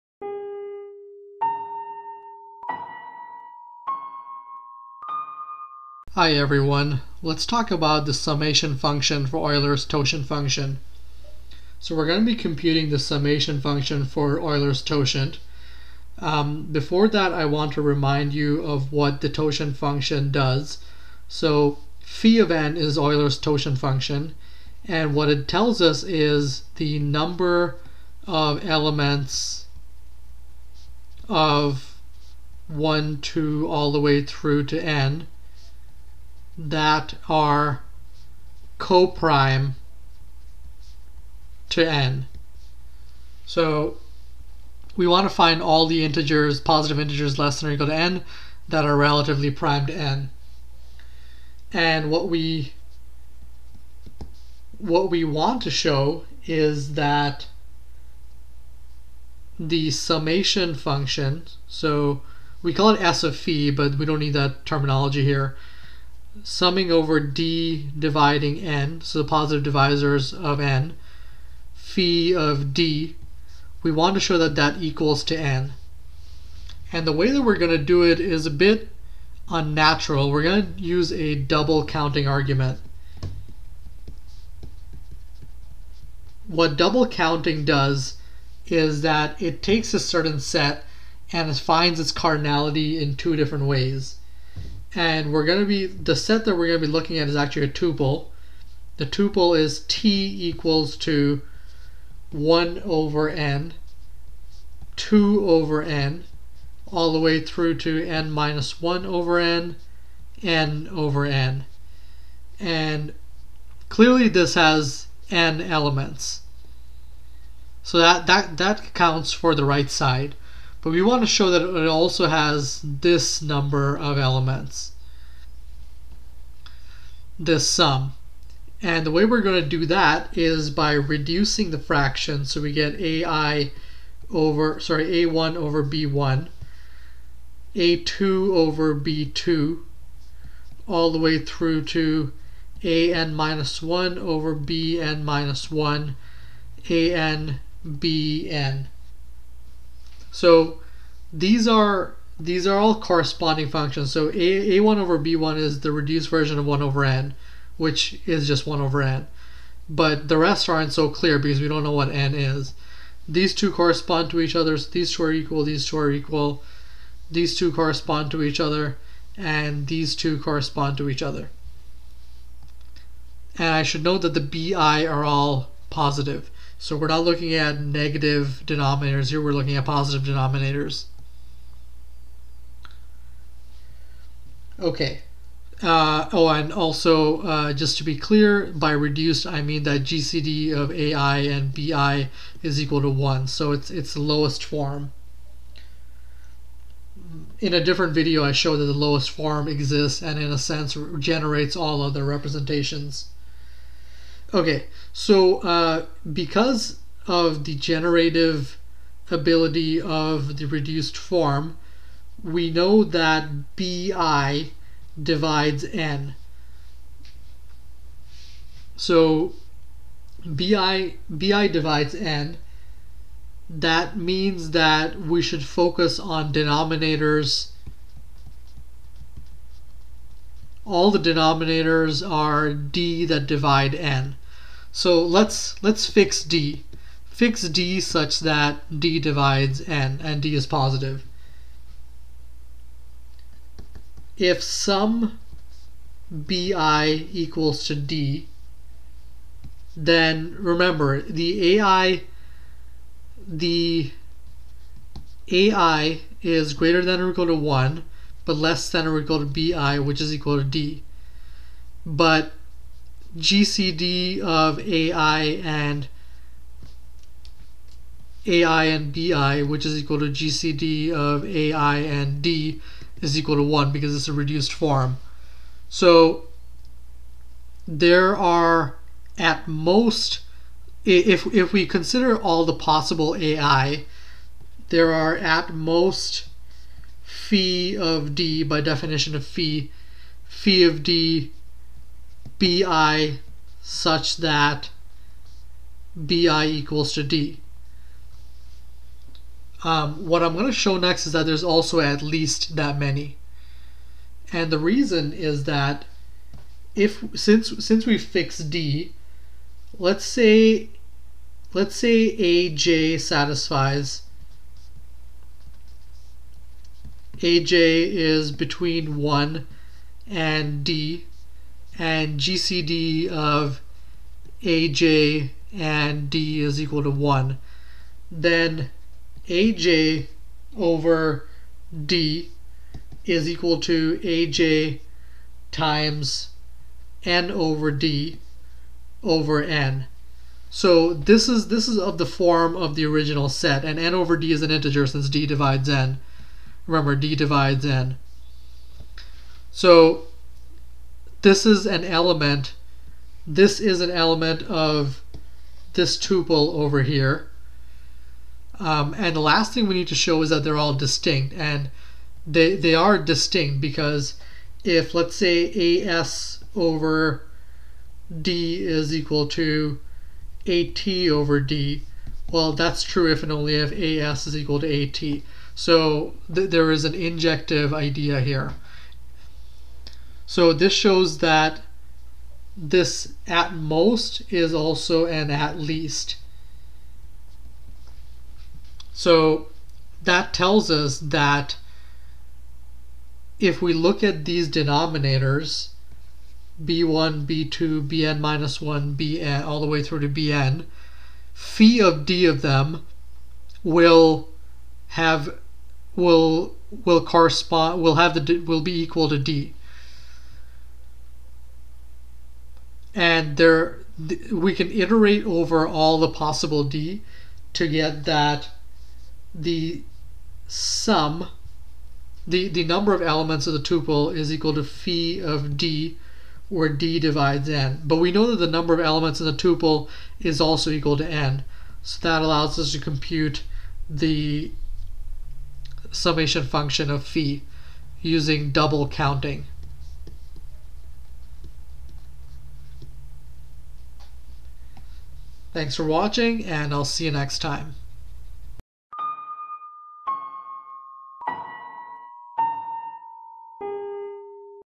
Summation Function for Euler's Totient Function
What do we get if we fix a positive integer, and take the sum of Euler's totient function applied to each positive divisor of the original integer? Incredibly, it returns the original integer. This means that the summation function of Euler's totient function is the identitiy function. We prove this fact using an out-of-the-blue double counting argument.